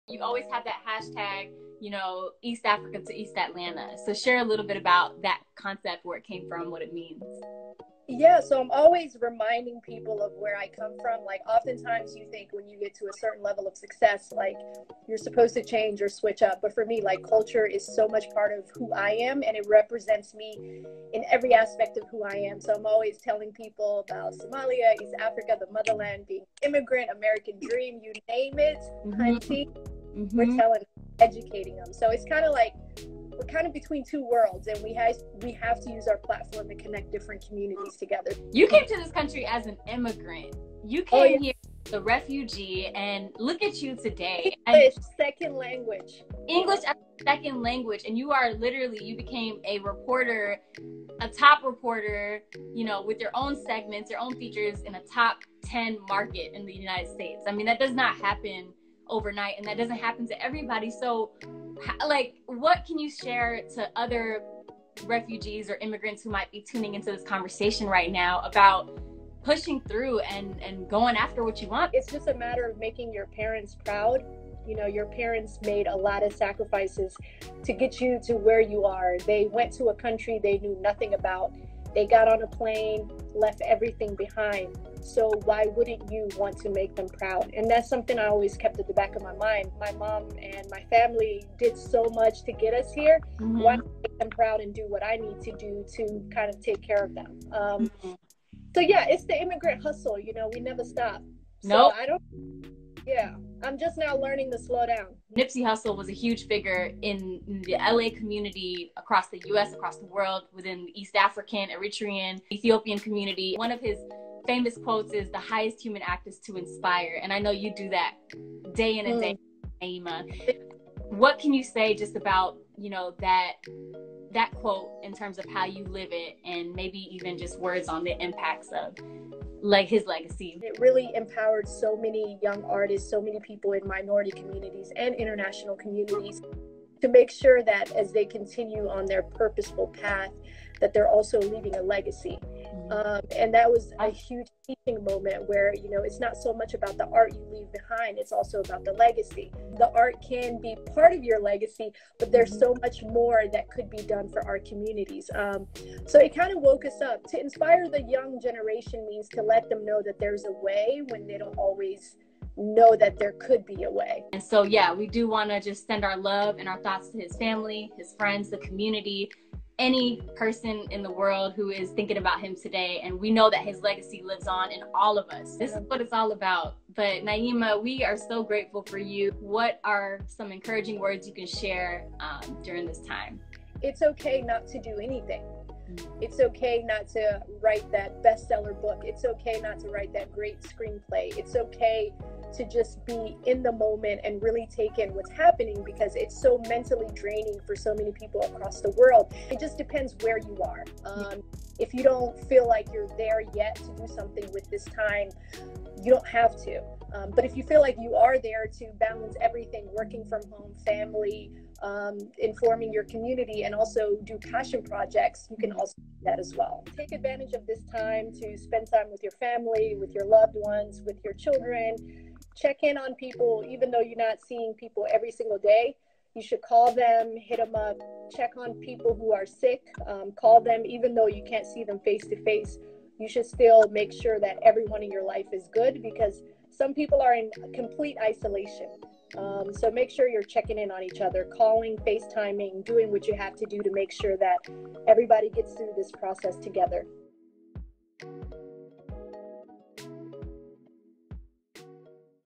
Dose of Positivity: “It’s ok not to do anything...”
With so much negativity going around the world, I hope these messages will brighten up your day and remind us to stay hopeful. During these tough times, we want to extend positive encouragement from awesome people around the world, share messages from their journeys and promote about safety and wellbeing. 💚💛❤️💙⁣

This Dose of Positivity features Neima Abdulahi, Emmy-nominated/award-winning journalist, culture content creator & hip-hop connoisseur, born in Somalia & raised in Atlanta.

These videos are captured live so make sure to follow HannahJoyTV on instagram to join the chat!